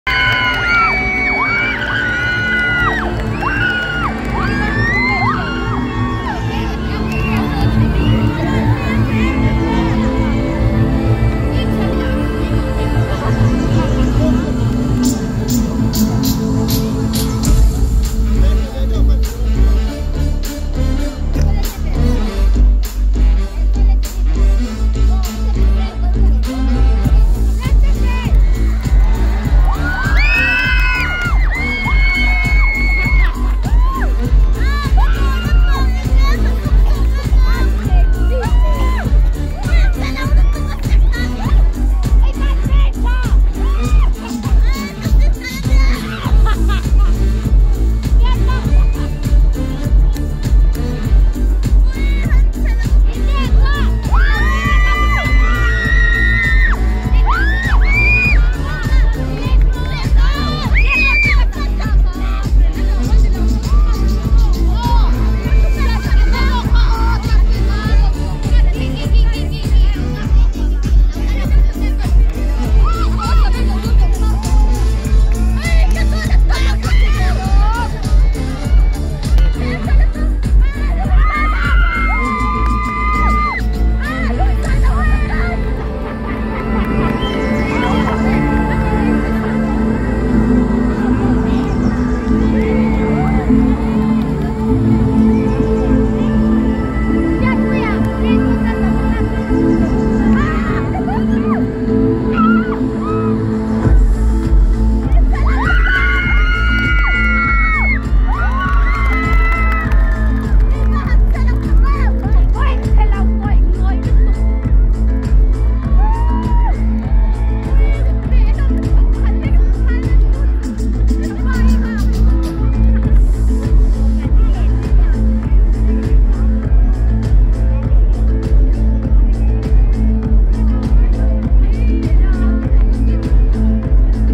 Chapchar Kut 2024 || Mizo Cultural Fashion Show || AR Ground
The Chapchar Kut is a festival of Mizoram, India. It is celebrated during March after completion of their most arduous task of jhum operation i.e., jungle-clearing (clearing of the remnants of burning). It is a spring festival celebrated with great favour and gaiety.